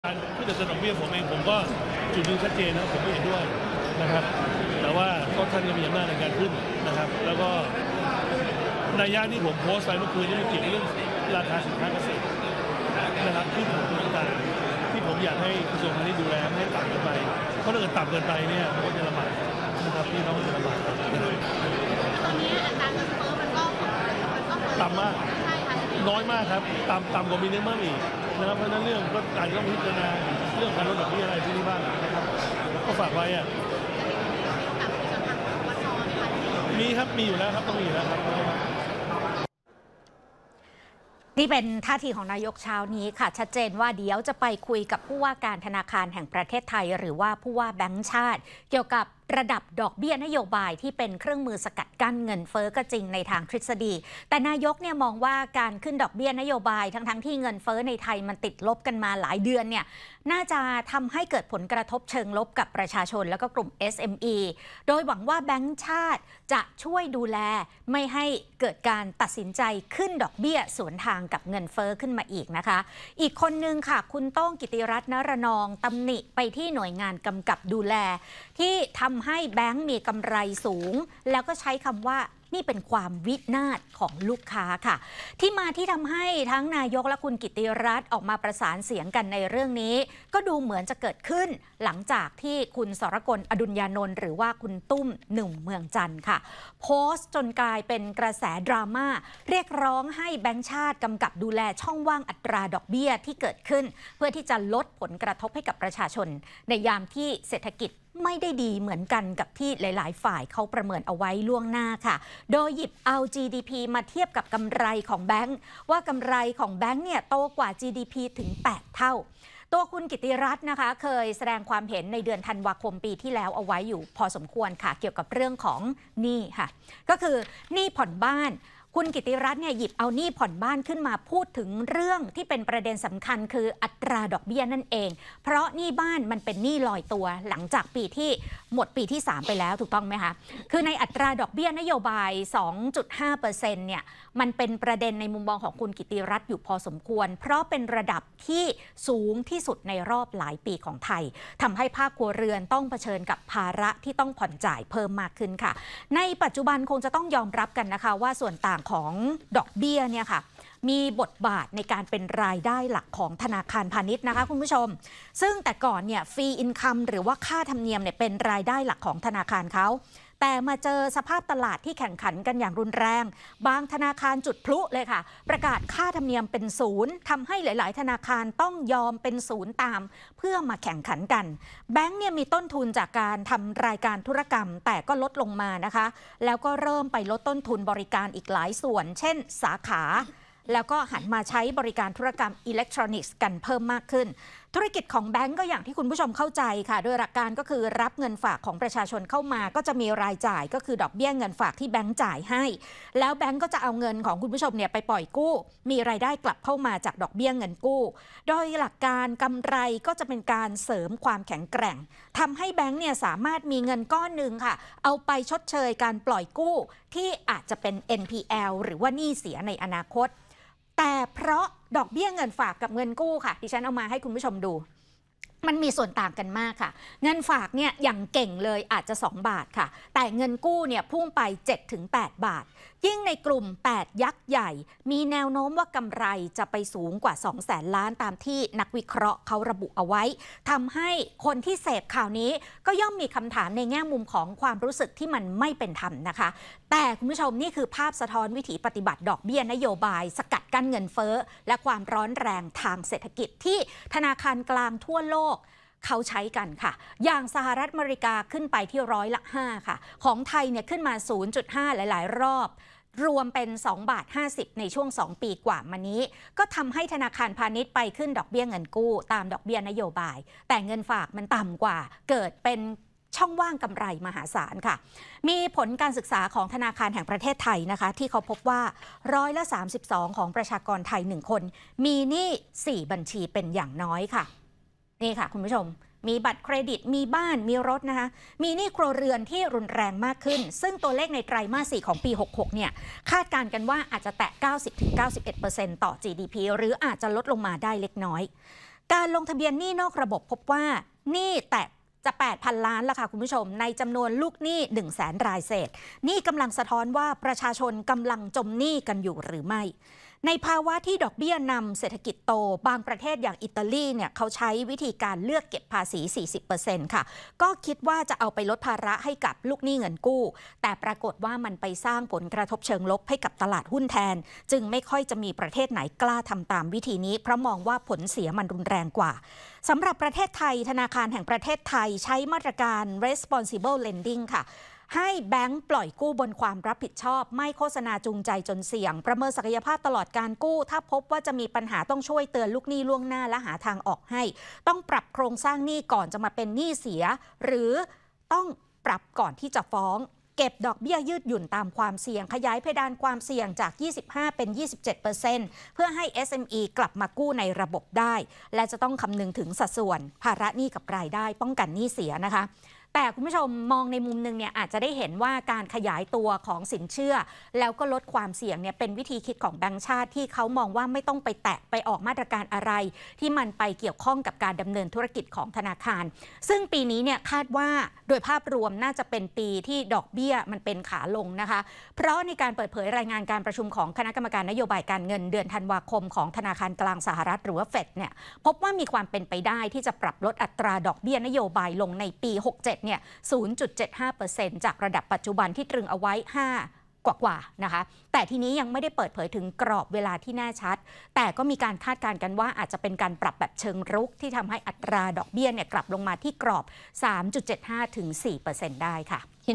ดอกเบี้ยขาขึ้น ธนาคารรวย คนผ่อนจมหนี้ ? | Thai PBS News
เป็นดรามาทัชใจคนดูข่าว หลังคุณตุ้ม สรกล อดุลยานนท์ เจ้าของนามปากกา "หนุ่ม เมืองจันท์" ออกมาแสดงความเห็นเกี่ยวกับผลกำไรของธนาคารพาณิชย์ ที่สวนทางกับสภาพเศรษฐกิจ เรียกร้องให้ธนาคารแห่งประเทศไทยควบคุม วันนี้มีปฏิกิริยาต่อเนื่องจากนายกรัฐมนตรี ซึ่งยังยืนยันในจุดยืนที่ไม่เห็นด้วยกับระดับดอกเบี้ยในเวลานี้ เพราะมองว่าเงินเฟ้อมันต่ำลงแล้ว